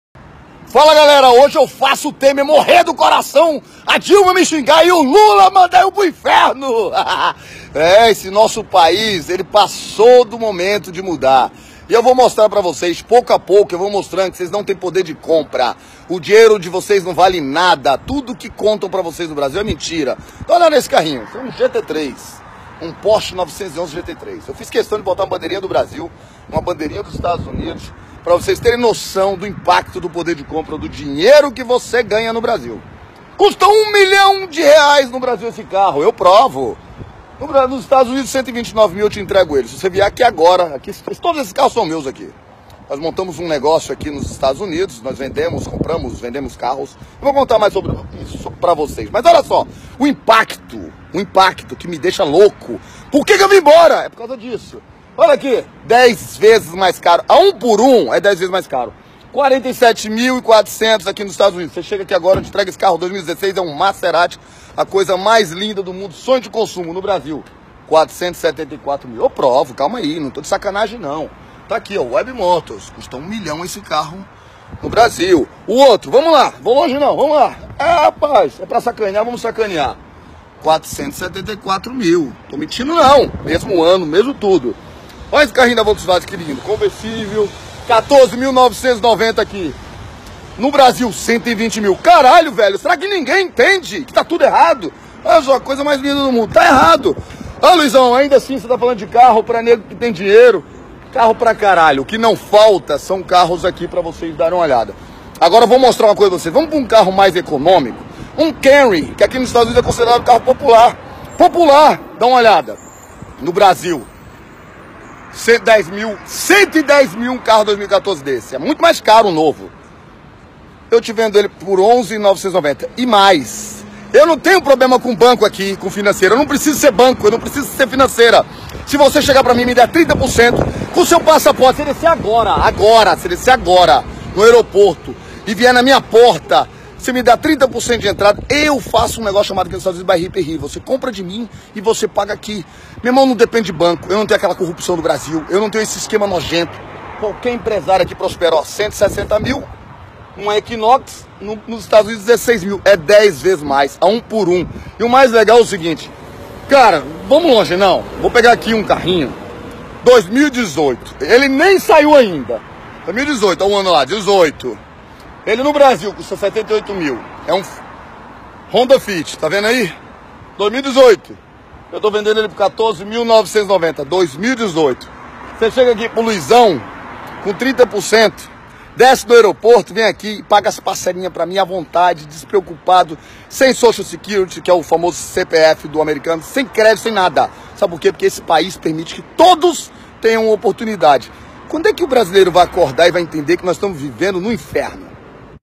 Carro PORSCHE por R$129 mil. NO BRASIL custa R$ 1 milhão.
Luiz Miranda expõe e critica política financeira brasileira: SOMOS ESCRAVIZADOS por um sistema opressor. Poder de compra falido.